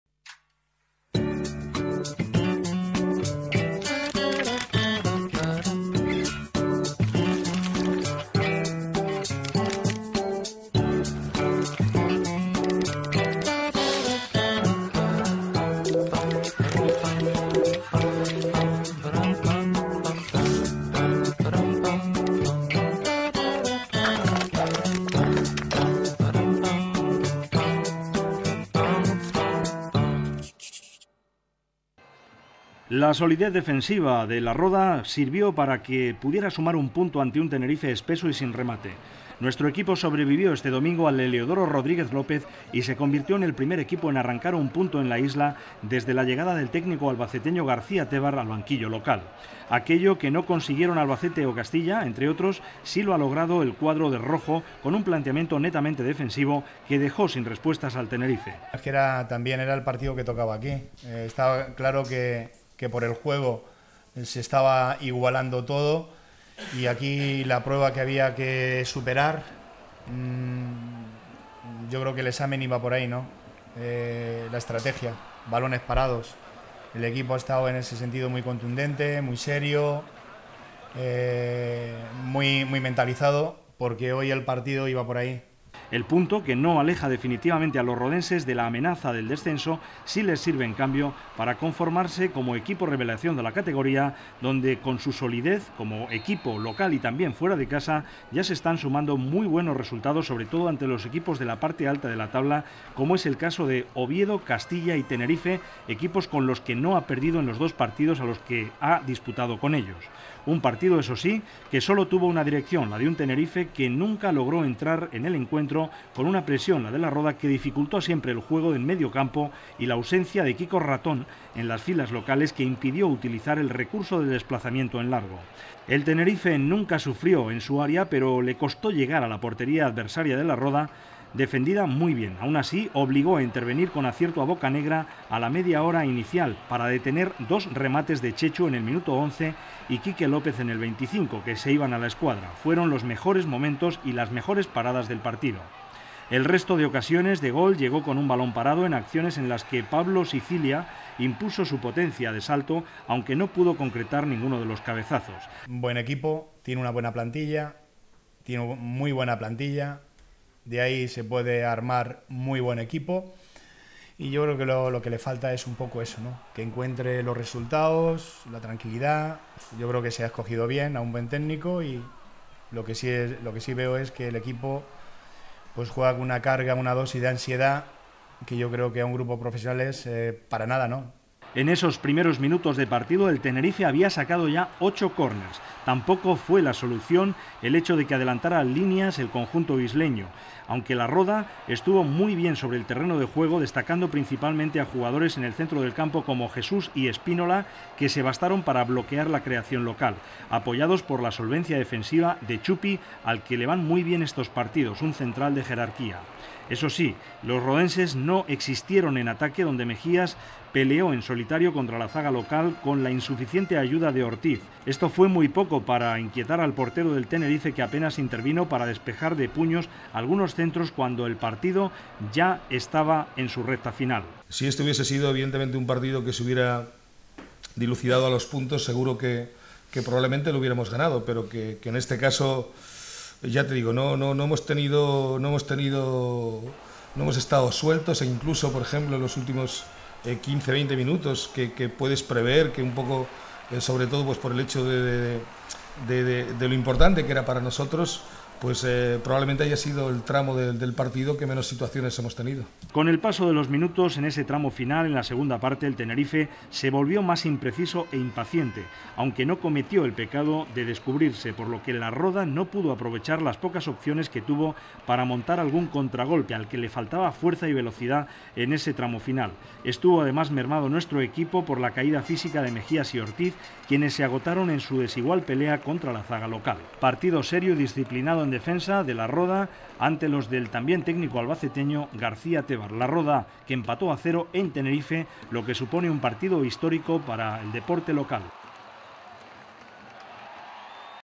La Roda frena al Tenerife (0-0)
La solidez defensiva del conjunto manchego le sirve para sumar un punto ante un Tenerife espeso y sin remate. La Roda sobrevivió este domingo al Heliodoro y se convirtió en el primer equipo en arrancar un punto en la Isla desde la llegada de García Tébar al banquillo local. Aquello que no consiguieron Albacete o Castilla, entre otros, sí lo logró el cuadro dirigido por Antonio Cabezuelo gracias a un planteamiento netamente defensivo, que dejó sin respuestas al CD Tenerife.